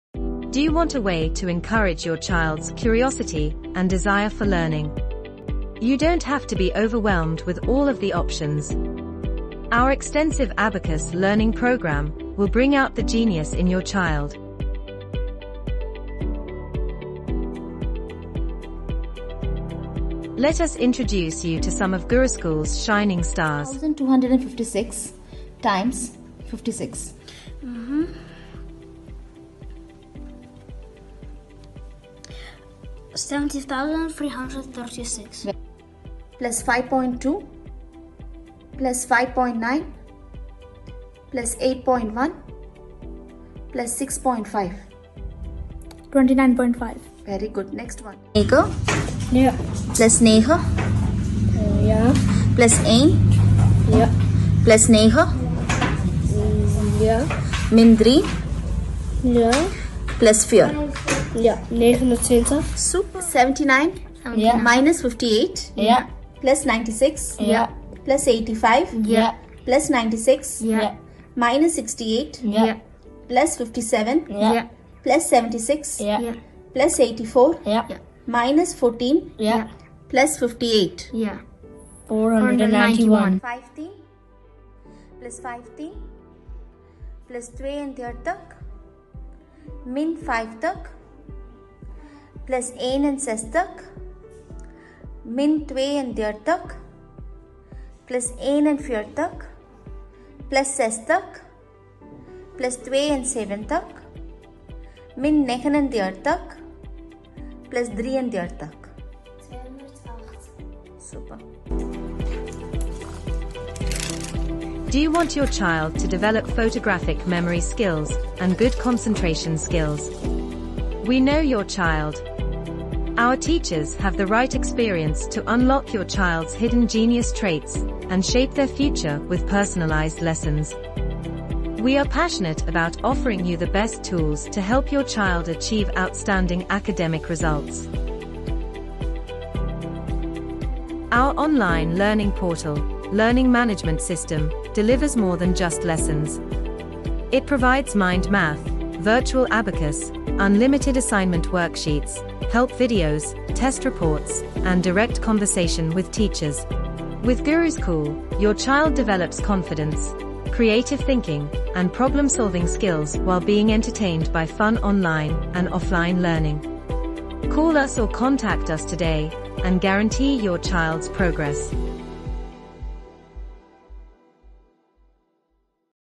Guruscool Abacus Math Training
Guruscool Abacus Math Training makes children of 5-12 years enjoy learning Math using Abacus visualization technique. Calculations based on Abacus was invented at ancient times and now widely used as brain development programme. Children do fast calculations in mind by visualizing Abacus bead movements. Researches proves that Abacus learners uses both sides of the brain- left hemisphere & right hemisphere and hence highly effective in improving the concentration and memory level in children. Children who learn Abacus can do calculations faster than a calculator apart from improving their learning skills, visualisation skills, concentration and memory skills.